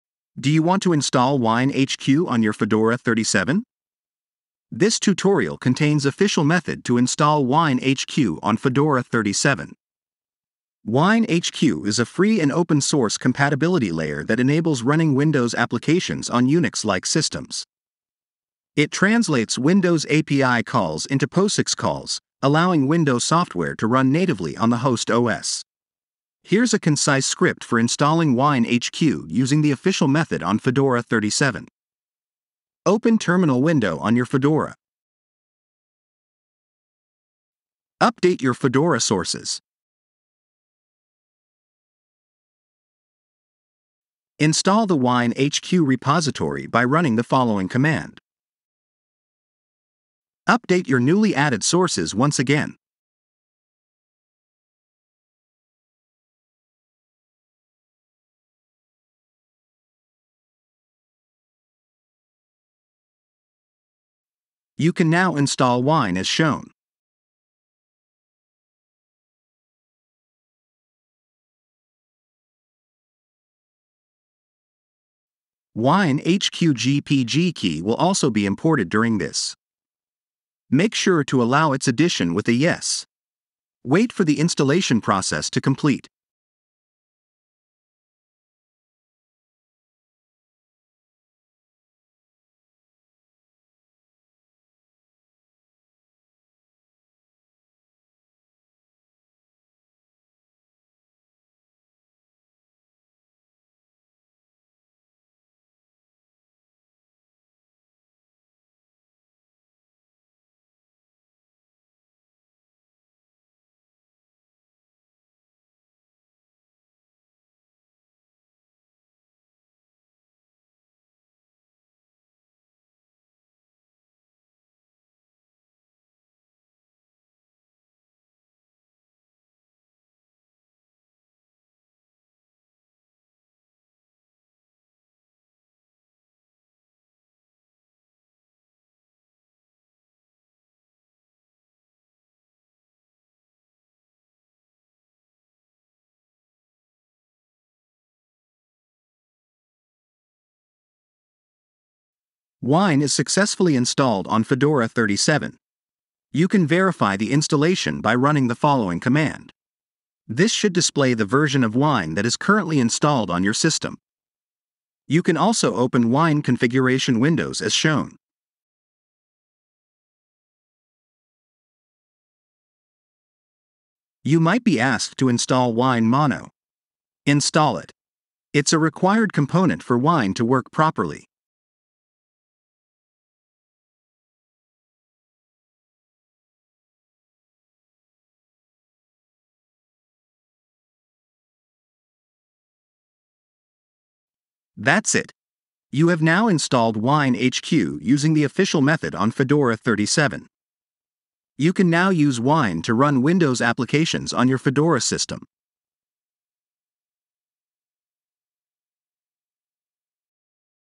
How to Install WineHQ 8 on Fedora 37 Linux WineHQ Official Method
How to Install WineHQ 8 on Fedora 37 Linux WineHQ Official Method - WineHQ is widely used by Linux and macOS users who need to run Windows applications for work, gaming, or other purposes, but do not want to switch to Windows as their primary operating system. Wine supports a large number of Windows applications and games, but not all software is compatible, and some programs may require additional configuration or tweaks to run properly.